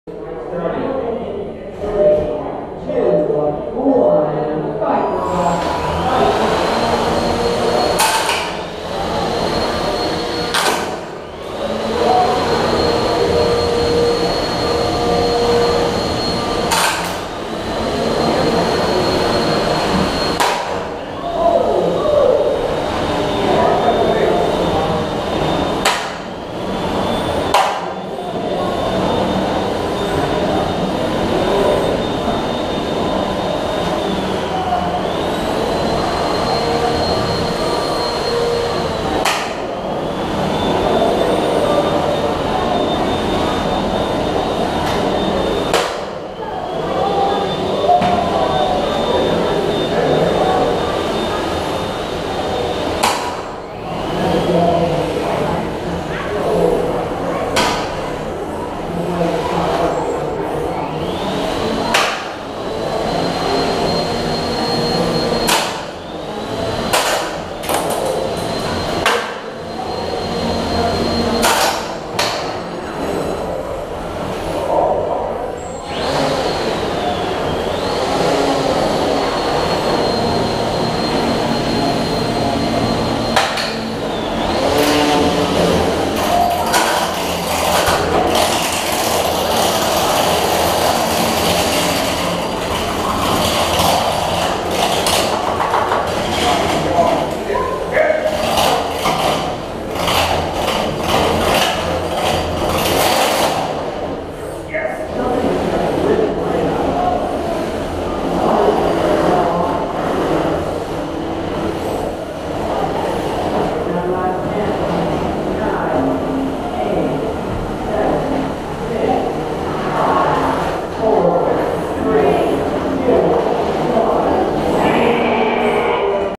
Clash of the Bots 2017 150g : The Mundane Rutabaga vs Nano Toxin v2
Clash of the Bots 2017 on June 24th 2017 at Gastonia, NC. Double Elimination format.

The Mundane Rutabaga : Red Square
Nano Toxin v2 : Blue Square